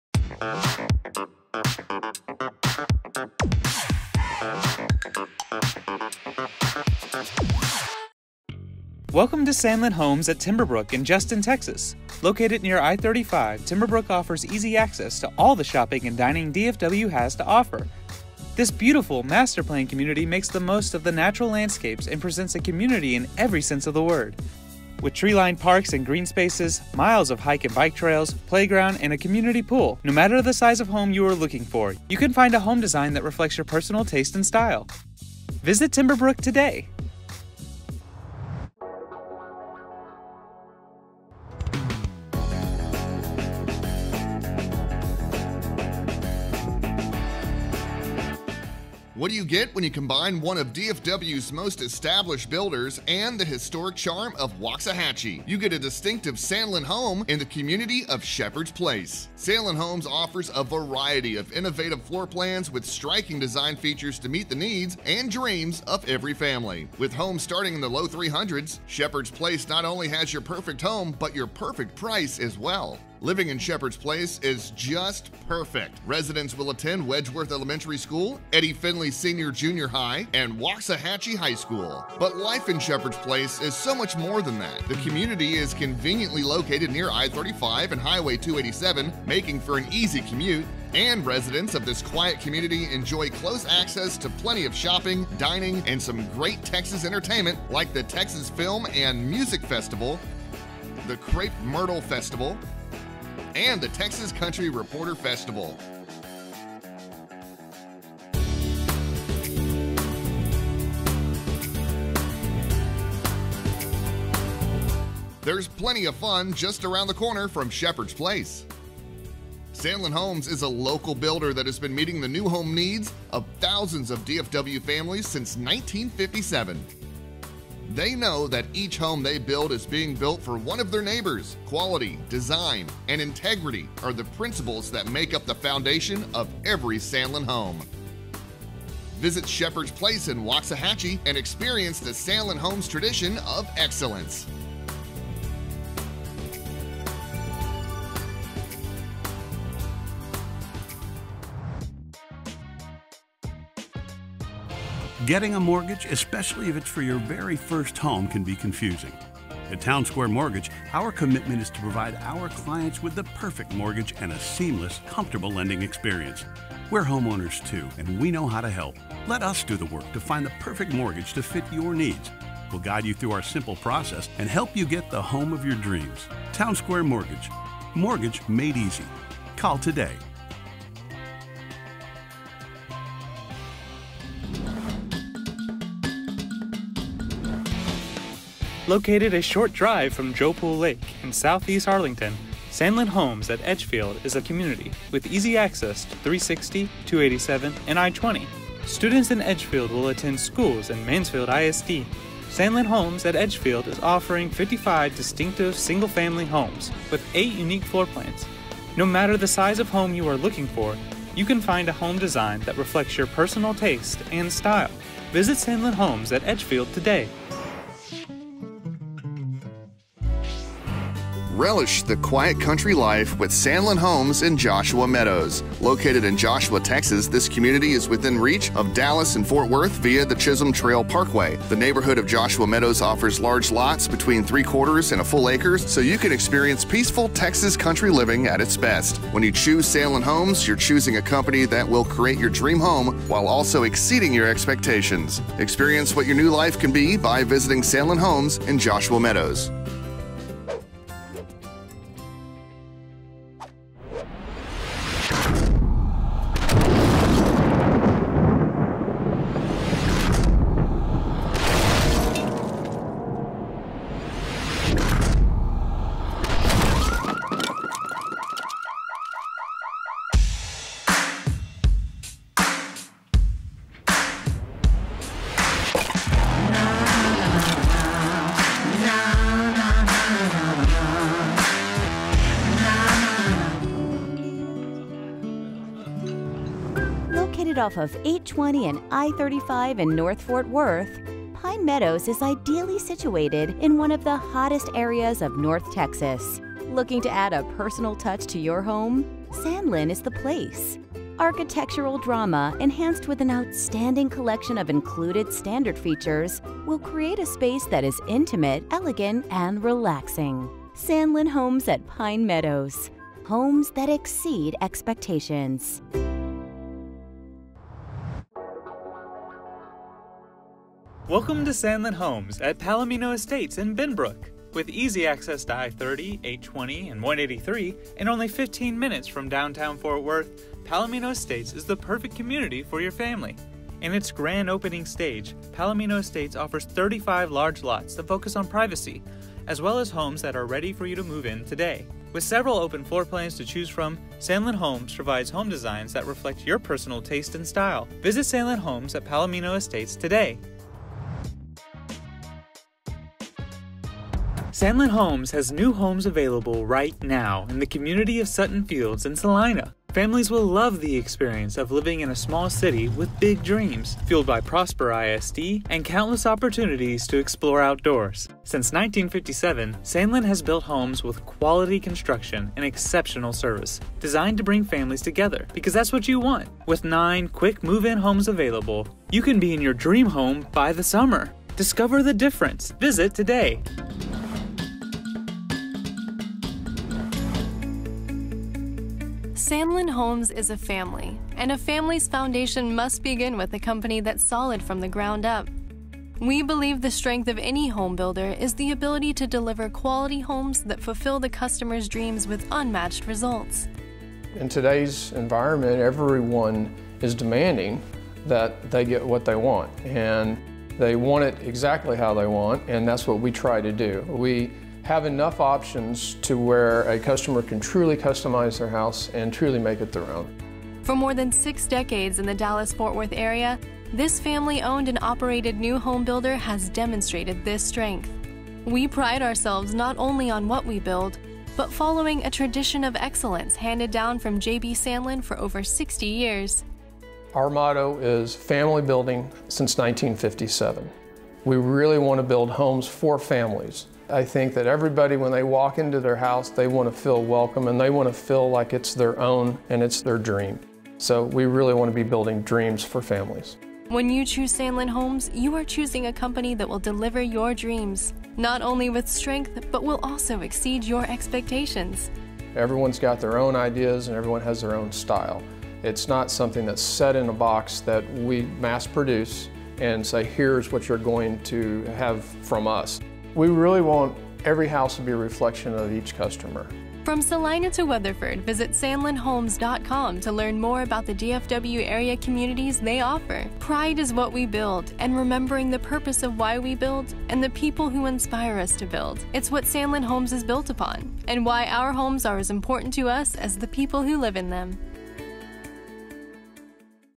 Sandlin Homes Communities in DFW Texas!
For more than six decades in Dallas-Fort Worth, the family-owned and operated home builder has demonstrated a tradition of excellence. When you choose Sandlin Homes, you are choosing a company that will create your dream home with strength and superiority while also exceeding your expectations.

Featuring:
Timberbrook
Sheppard's Place
Edgefield
Joshua Meadows
Pine Meadows
Palomino Estates
Sutton Fields
Sandlin Homes - Family Building Since 1957
Acme Brick
Town Square Mortgage